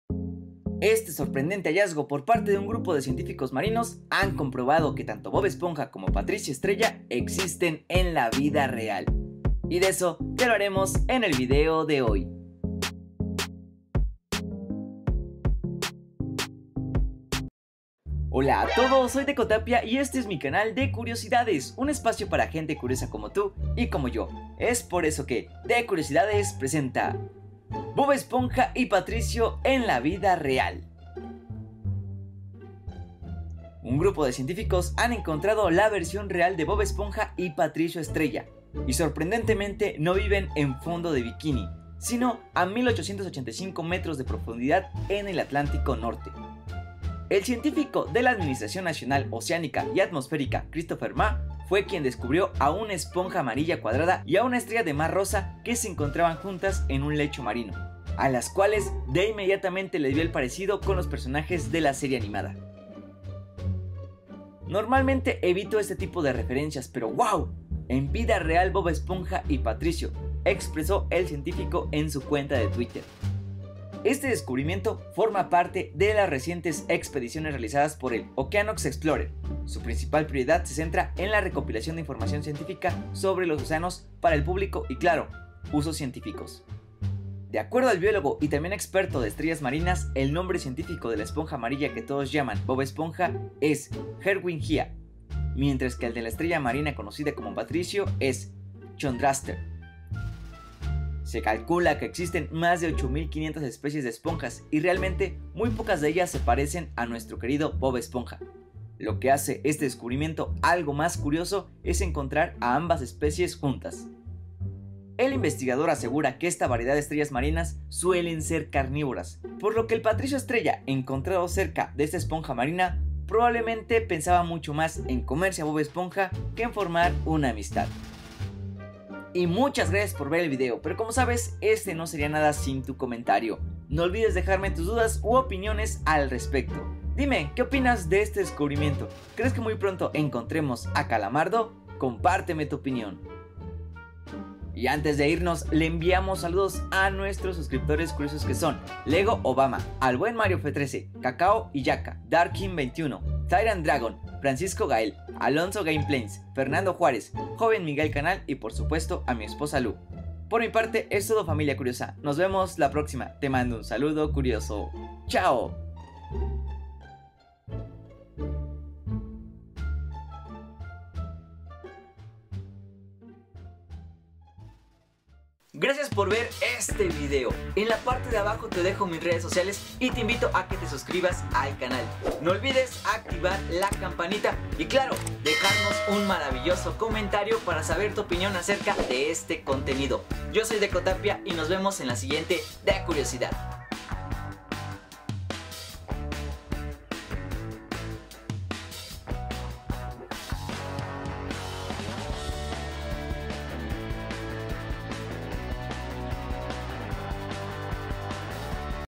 DESCUBREN a | BOB ESPONJA Y PATRICIO | en la VIDA REAL
Sorprendente Hallazgo por parte de un grupo de  científicos marinos, y es que han comprobado  que tanto “Bob Esponja” como  “Patricio Estrella” existen en la vida real, y de eso te hablaremos en el video de hoy.